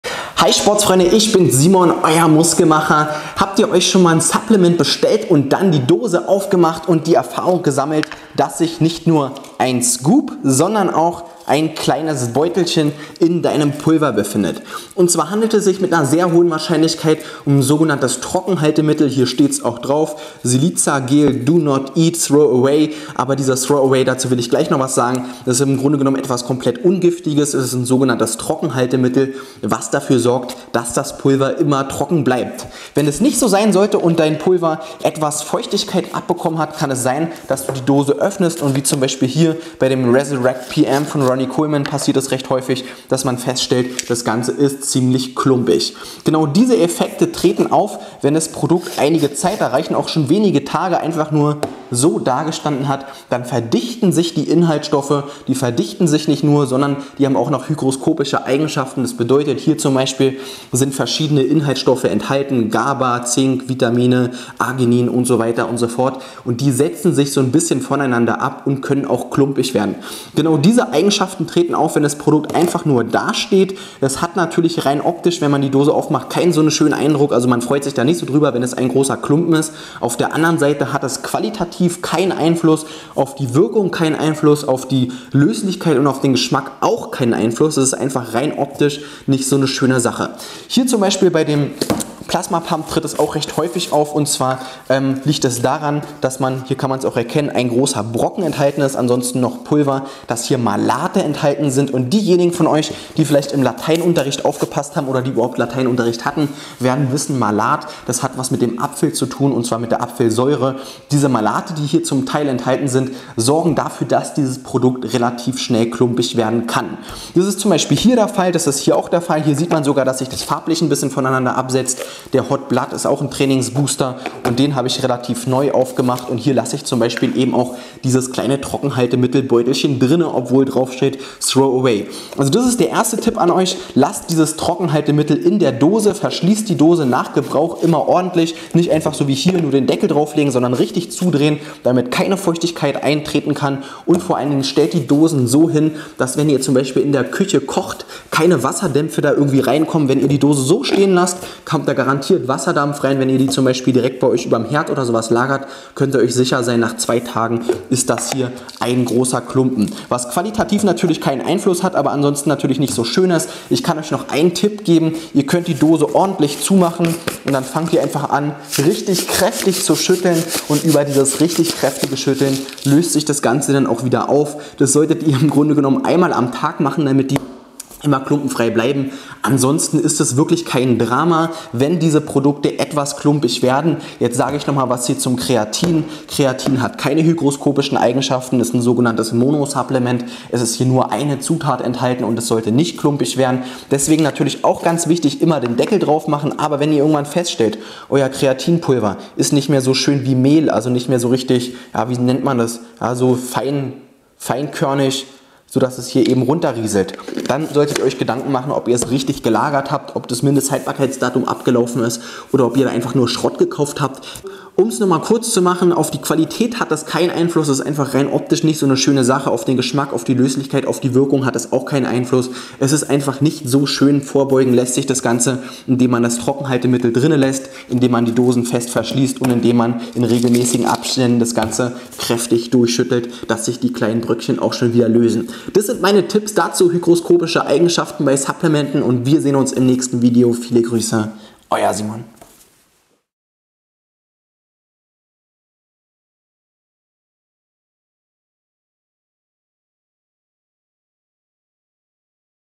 Hygroskopische Supplemente - Klumpige Pulver verhindern
Habt ihr schon Erfahrungen mit klumpigen Supplementen gemacht? Nachdem das ein oder andere Produkt geöffnet wurde, hat es sich schnell verdichtet und ist fest wie Beton oder klumpig wie Kloßbrühe geworden. In diesem Video erkläre ich euch was es mit hygroskopischen Supplementen auf sich hat und wie ihr klumpiges Pulver mit schlechter Löslichkeit verhindern könnt.